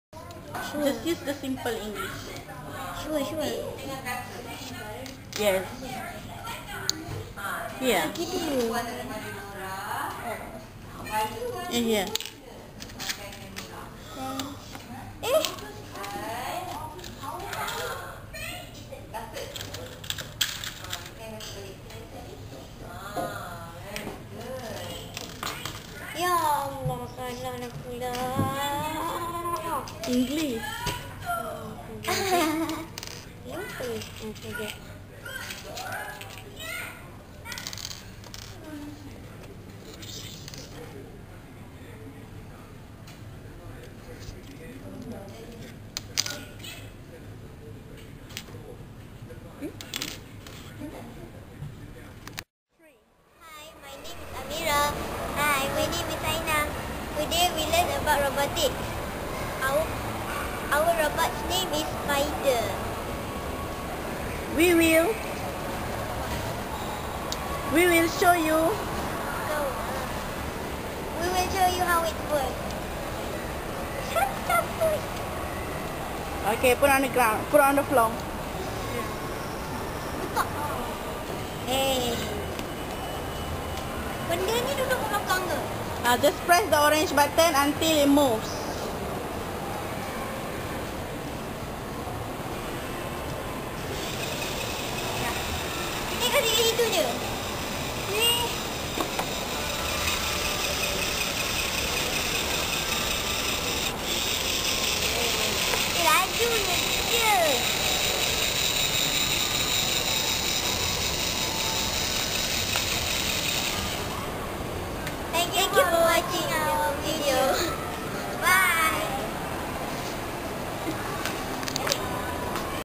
Robotics Spider by Amira Afiqah Hishamuddin and Aina Sofea Hisha
Student learnt about the basic capabilities of robots, learnt how to build simple robots using the Lego NXT kits, and program them to overcome simple challenges. Students are exposed to more challenging building and programming challenges that can be solved in future classes.